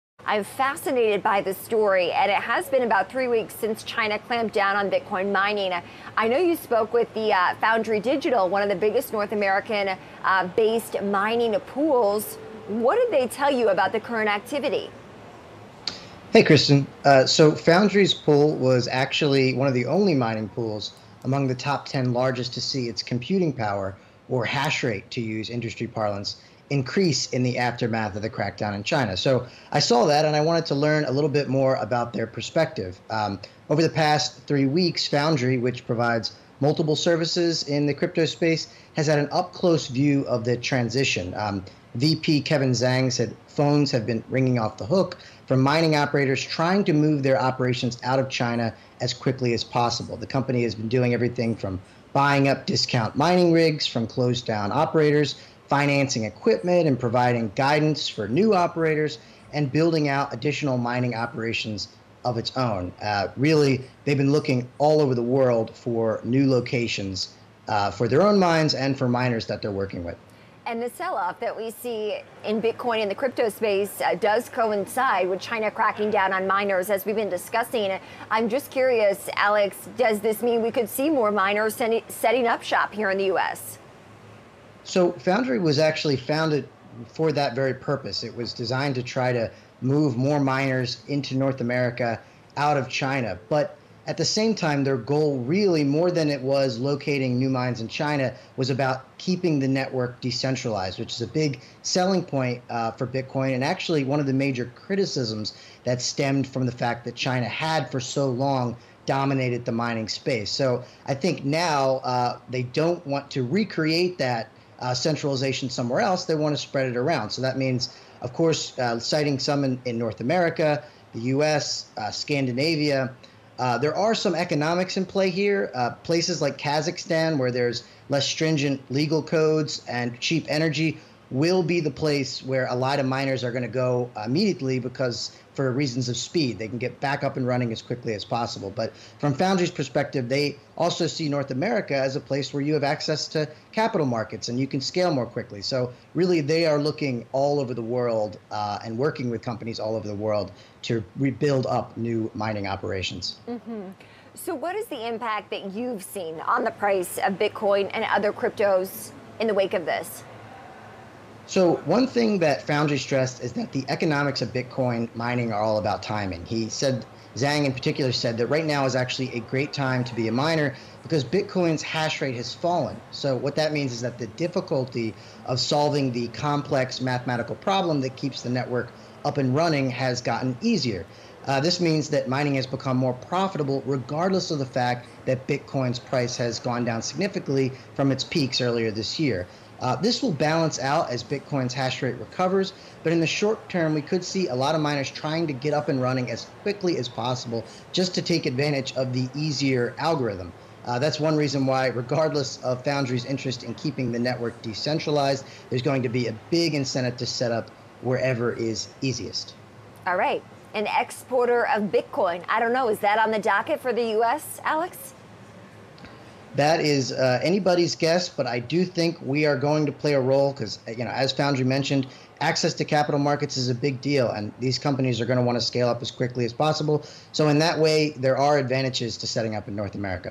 North American Bitcoin Mining Pool Reaps Benefits of a Post-China Future
Now that China, the world's second-largest superpower, is getting out of the crypto mining business entirely, at least one North America-based mining pool is reaping the benefits.

Foundry USA Pool, a wholly-owned subsidiary of the crypto-focused conglomerate Digital Currency Group, was founded with the goal of developing mines in North America. So when China brought down the regulatory hammer in May, it was already in a strong position to start rebuilding.